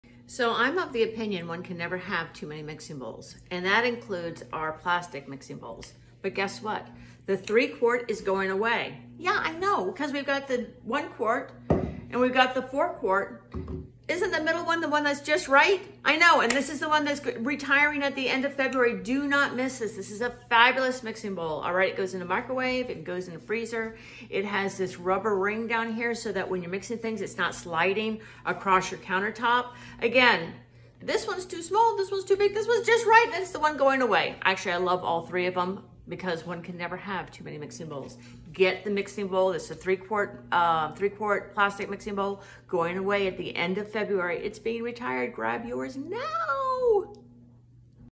The Pampered Chef 3-qt Plastic Mixing Bowl is retiring!
The 3qt Plastic Mixing Bowl is retiring at the end of February! Both microwave and freezer safe, this is the perfect size for mixing up all those yummy treats your family loves! The rubber ring on the bottom prevent it from skidding across the countertop - and onto the floor 🙄! You'll love this!